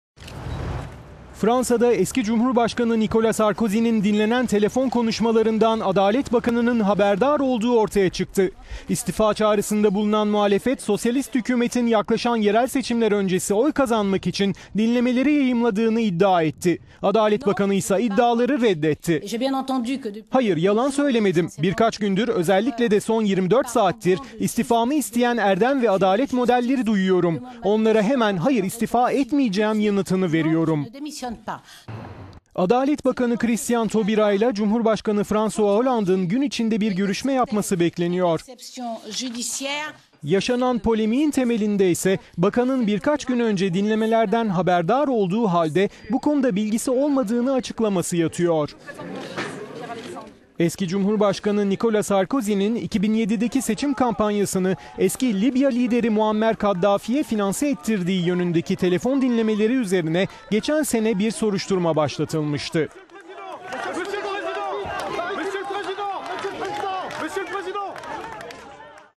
Sarkozy'nin telefon dinlemeleri hükümeti zora soktu
Fransa'da eski Cumhurbaşkanı Nicolas Sarkozy'nin dinlenen telefon konuşmalarından Adalet Bakanı'nın haberdar olduğu ortaya çıktı. İstifa çağrısında bulunan muhalefet, Sosyalist hükümetin yaklaşan yerel seçimler öncesi oy kazanmak için dinlemeleri yayımladığını iddia etti. Adalet Bakanı ise iddiaları reddetti:

Christiane Taubira:
"Hayır, yalan söylemedim. Birkaç gündür, özellikle de son 24 saattir istifamı isteyen erdem ve adalet modelleri duyuyorum. Onlara hemen, hayır istifa etmeyeceğim yanıt...